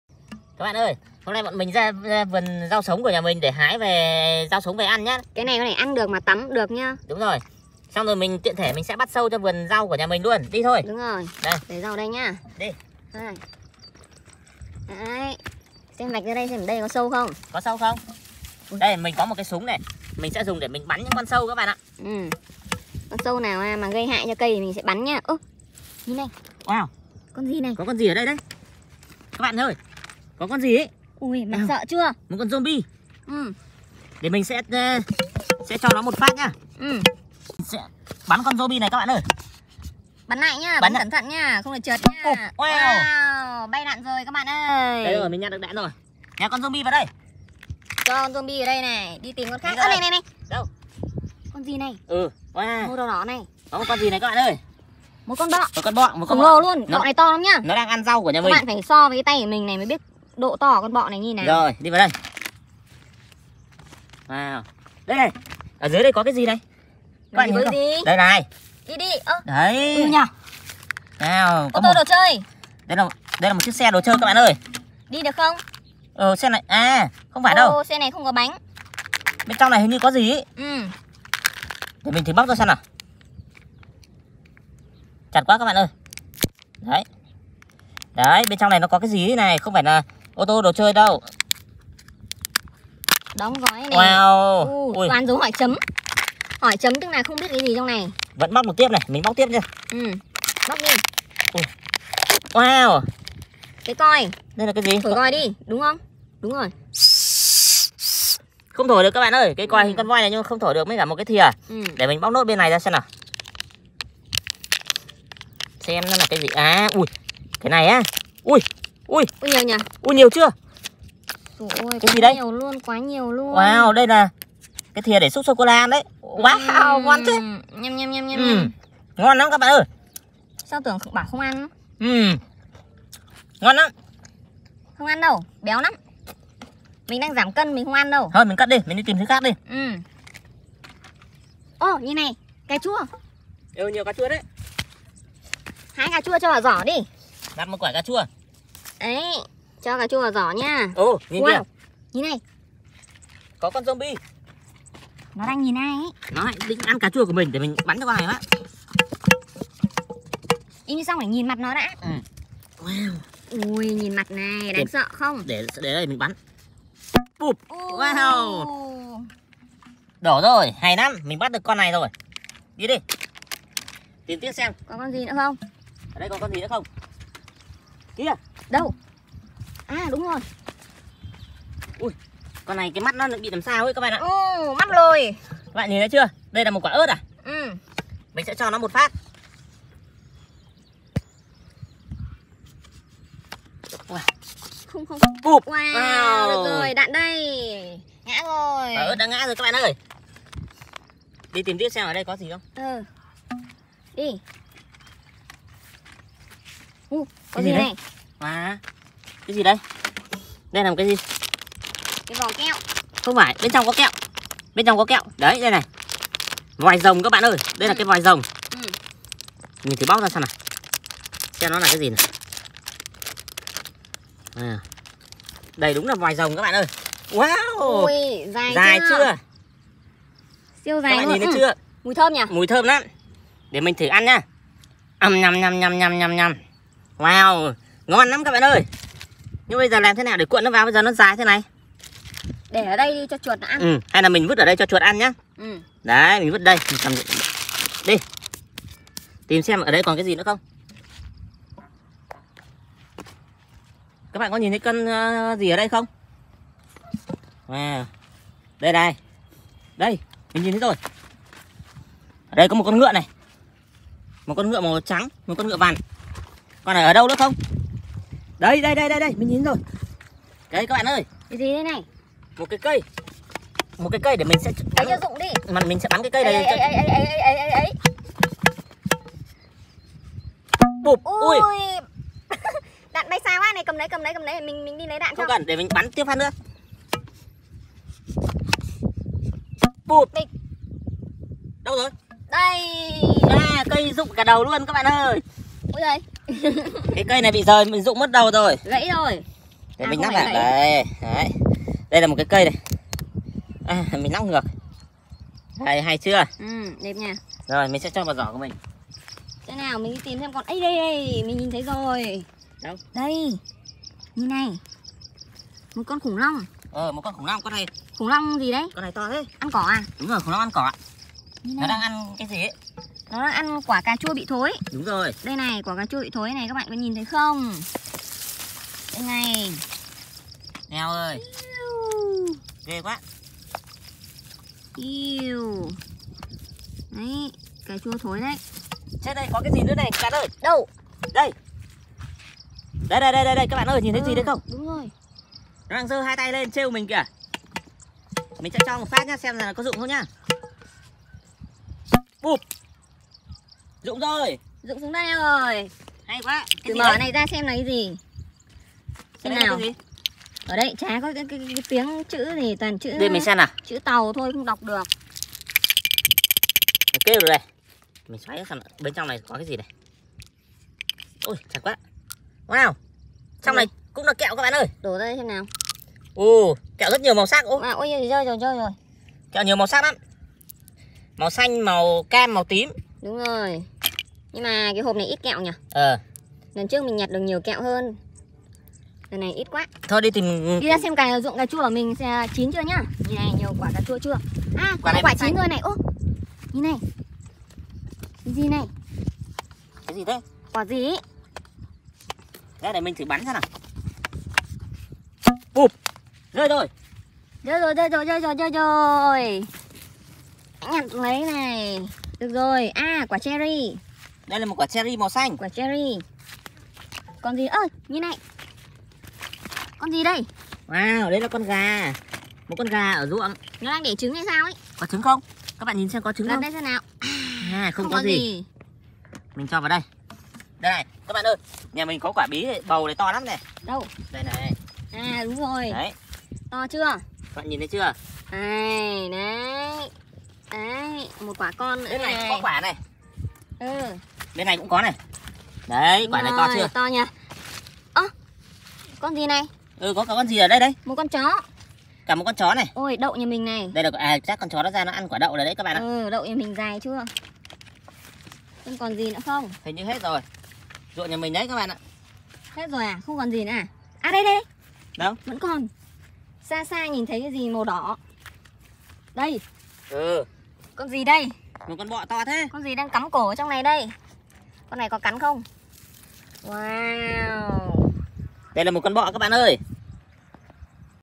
Trò chơi săn tìm zombie, các con vật đồ chơi và hoa quả
Tựa theo game plants vs zombies, chúng mình cùng chơi một trò chơi săn tìm các con zombie và hoa quả. Bọn mình sẽ dùng một cây súng đồ chơi để tìm các con vật đồ chơi và zombie ngoài ruộng nhé